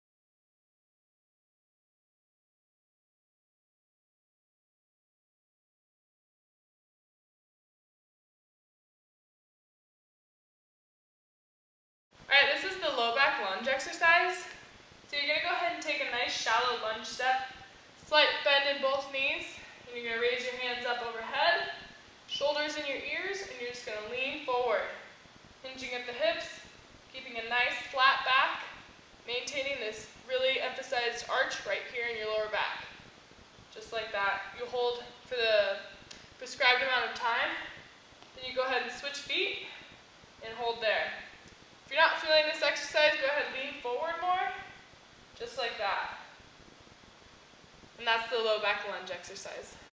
Low Back Lunge
Take a shallow lunge step with a slight bend in both knees. Arch your lower back and lean forward at the hips with hands locked out overhead palms facing each other. Hold for prescribed time and then switch feet.